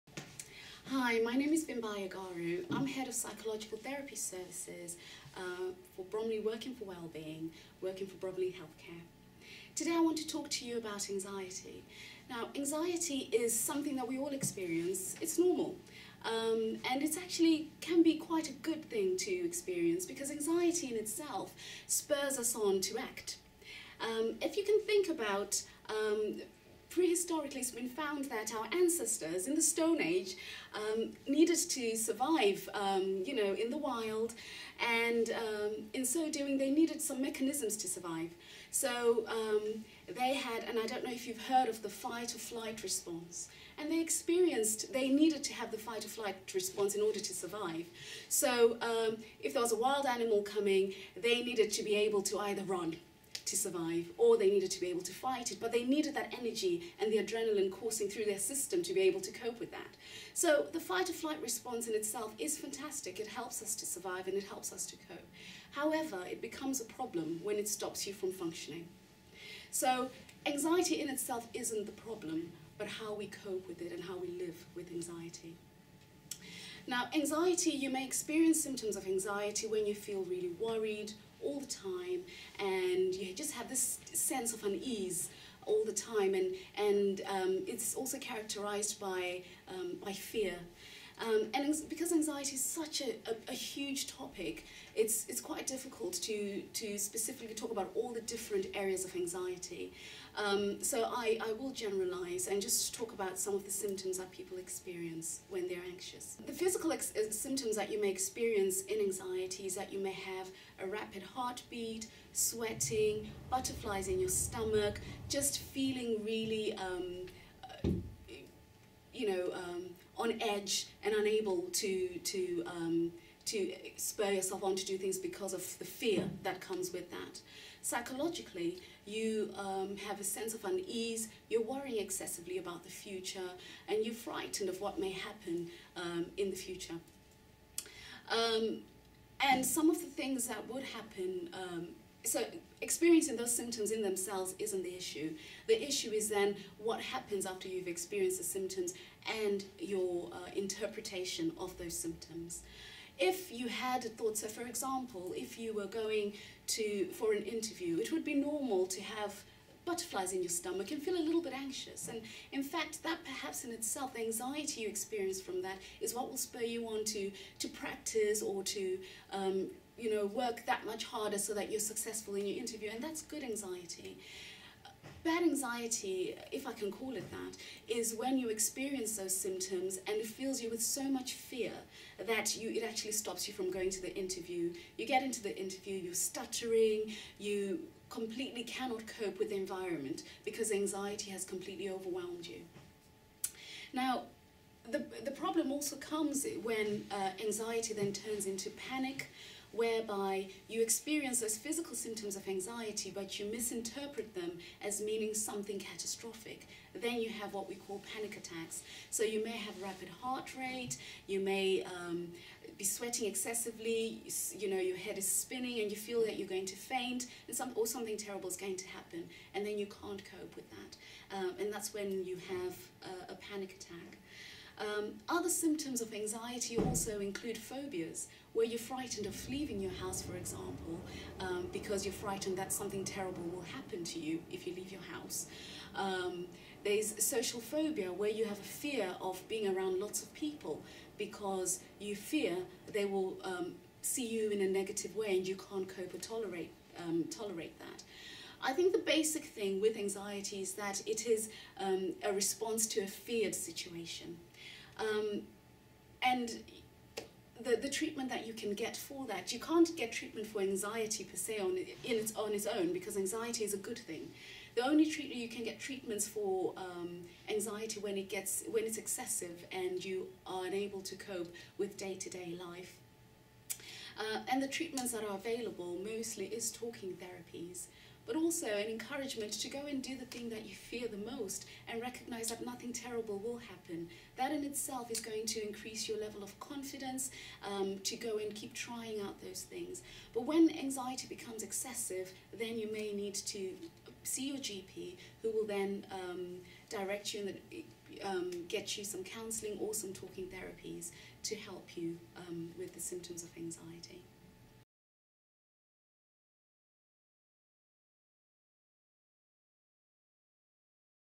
Anxiety
Vimbai talks about the causes of anxiety and how to cope with anxiety